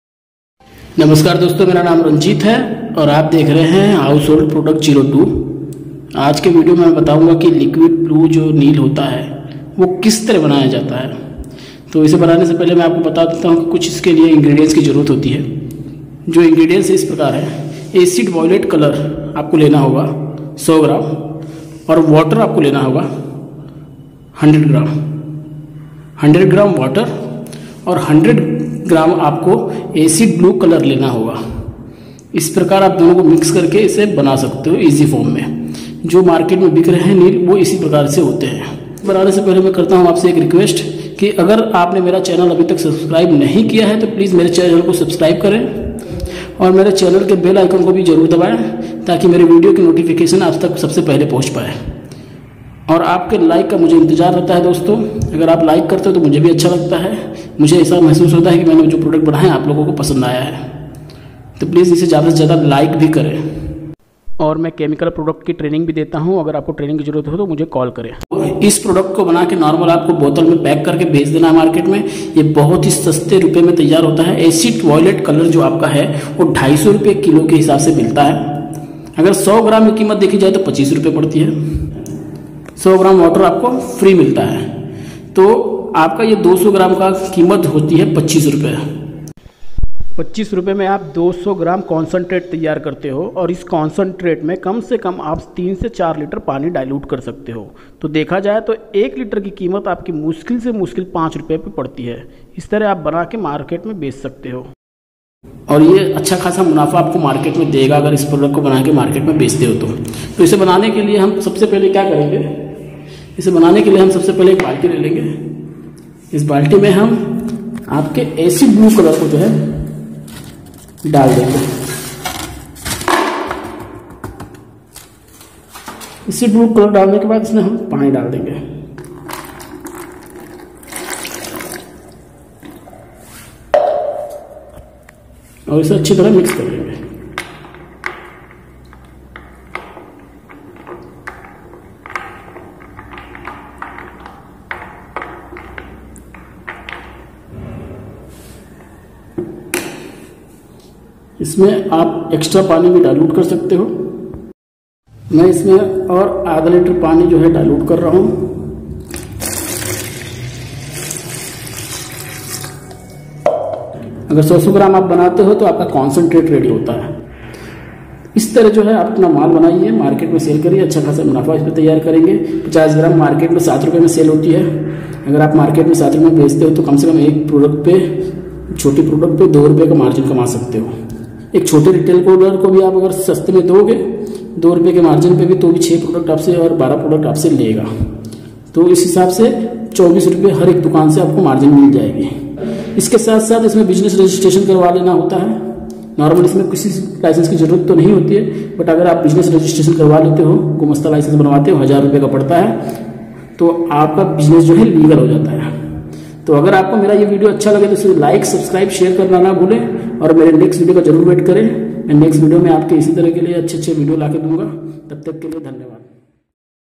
Neel banaye Ghar se || business idea small
Only for bussiness
This video given information about Neel.
Formula is as follows

Water 100 GM

Acid violet colour. 100 GM

After that mix 5 liter water

Your Neel is ready to sell in market.

Like my Facebook page. Household product02

Thanks.